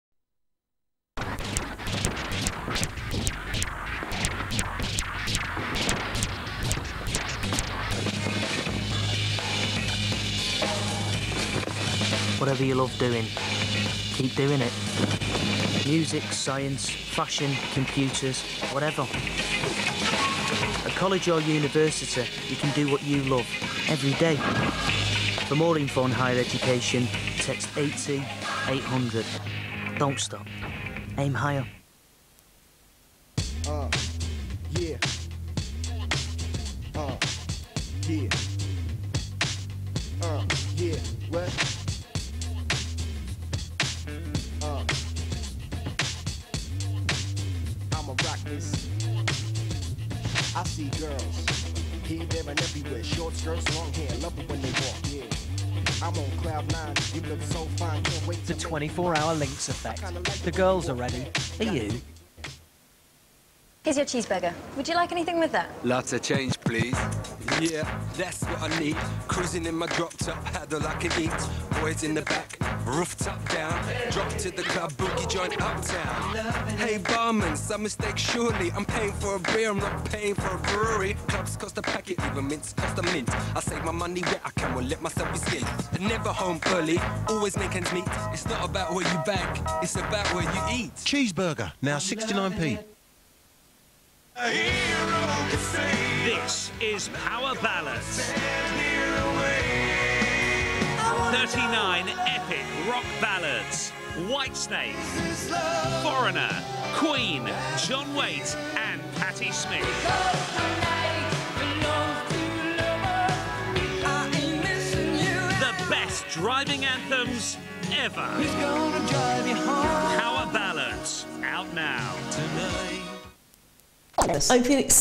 ITV1 Continuity: Early 00s
Not much continuity I'm afraid, there's more on this tape but stuff from channel 4 and ITV from the mid 2000's